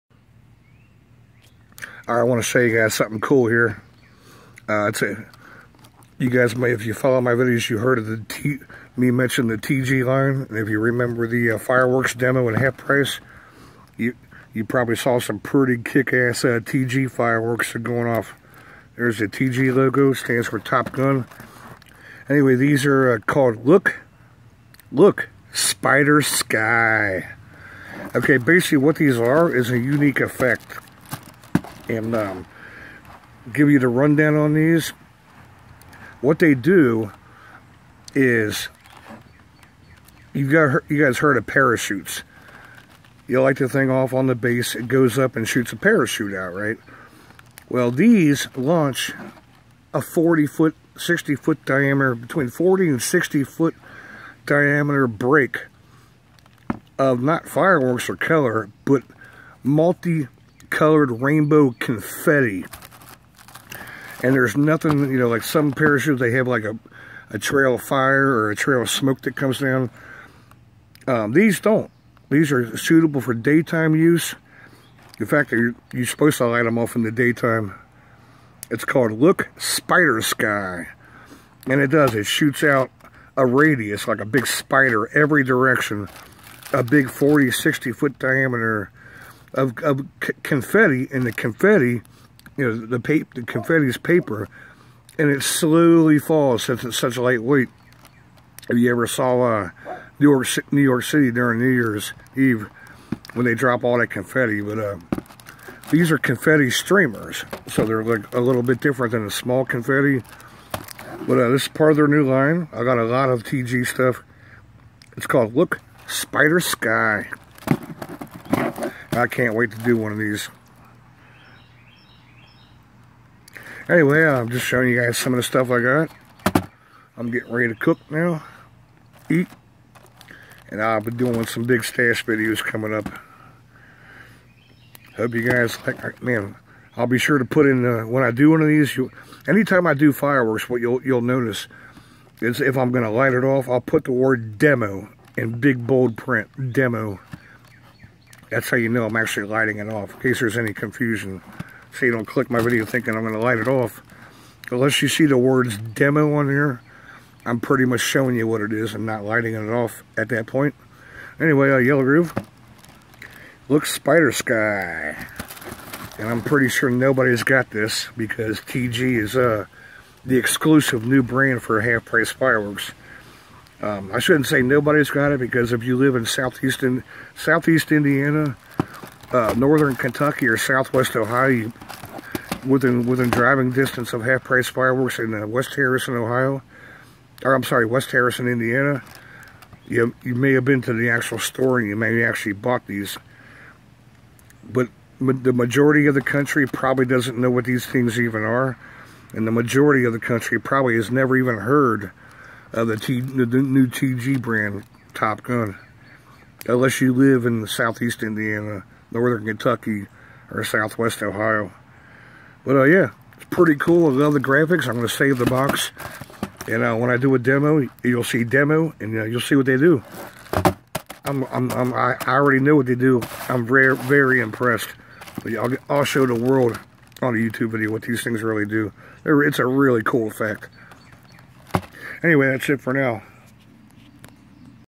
New TG brand, ( Top Gun) Look, Spider Sky, single shot Tubes
These have a unique effect, they shoot out a 40-60 foot dia break of colorful paper streamers ( confetti) in a 360 degree pattern that mimics the way a spider looks , and the way spider legs look ! Very cool effect not seen before around here. *** alert*** i have been told this new version is way better than the 1st gen of this same item, cant wait to demo it. need a day with no wind.